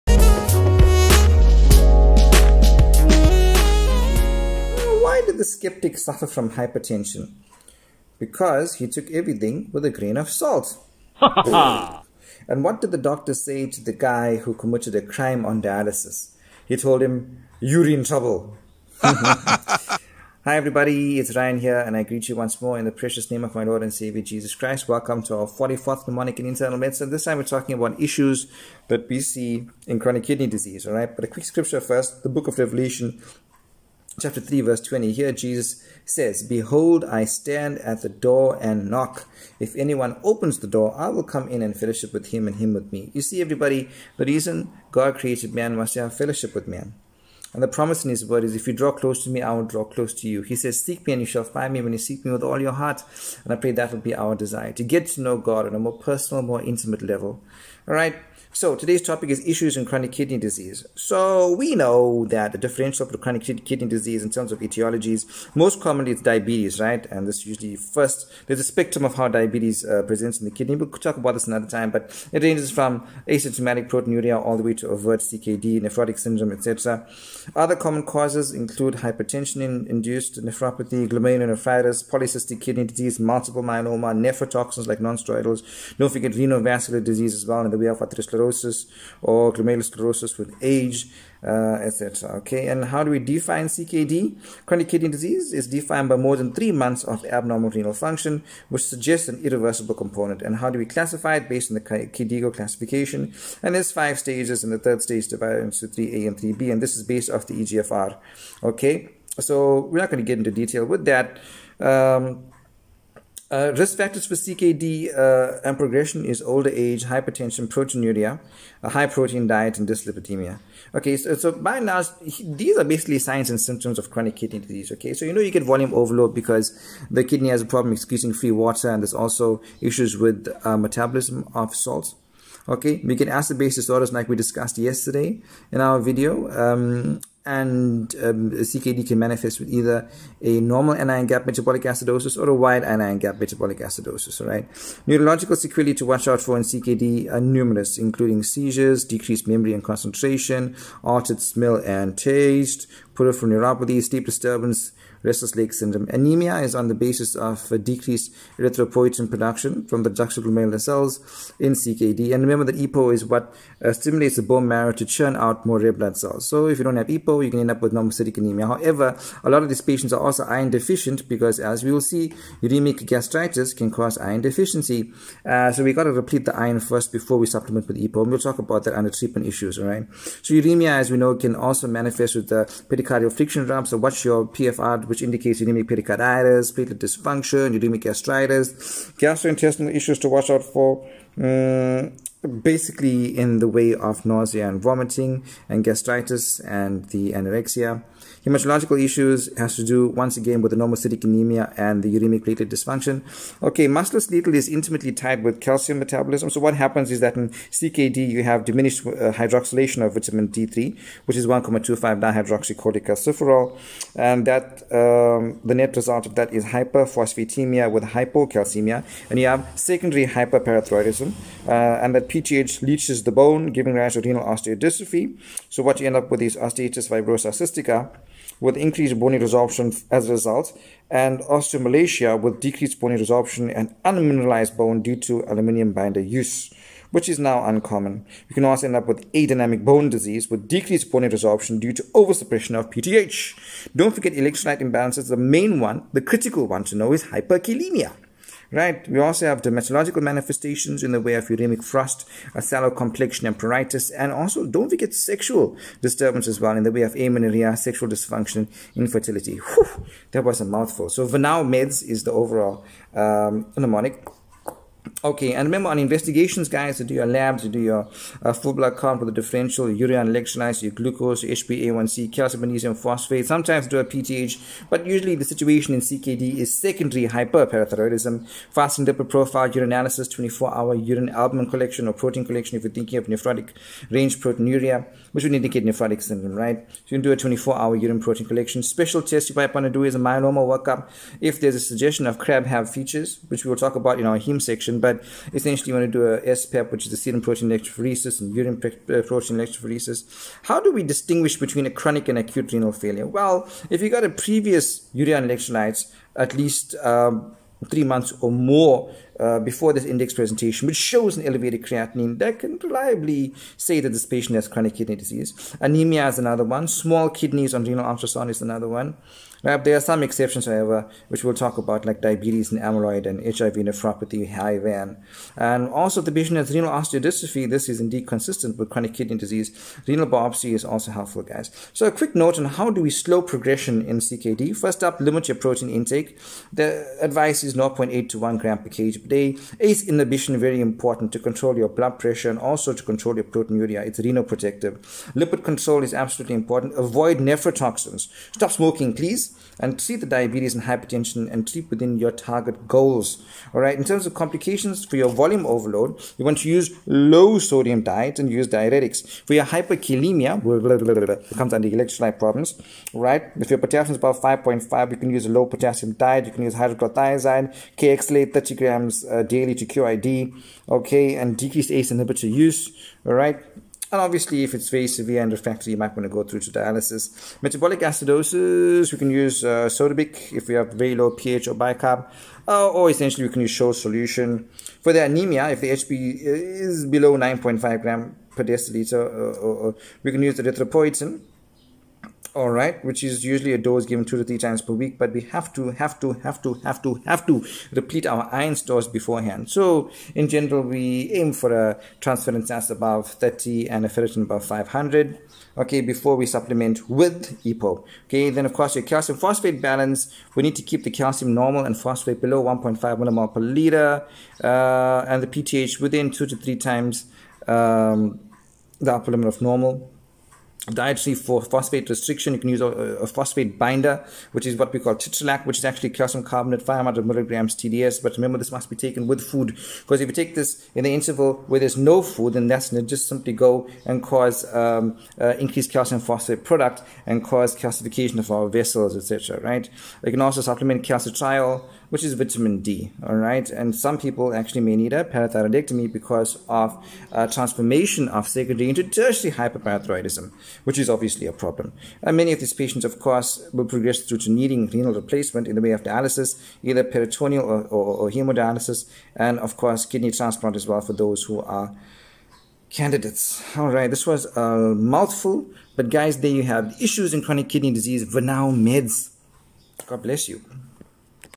Mnemonic 44 : Issues in Chronic Kidney Disease
Greetings in the powerful name of Jesus!
I hope you and your family are well

Thanks once more for joining me in another video
Here, we are exploring the features of chronic kidney disease and an easy way to remember them in mnemonic fashion

I trust you find this helpful

God bless!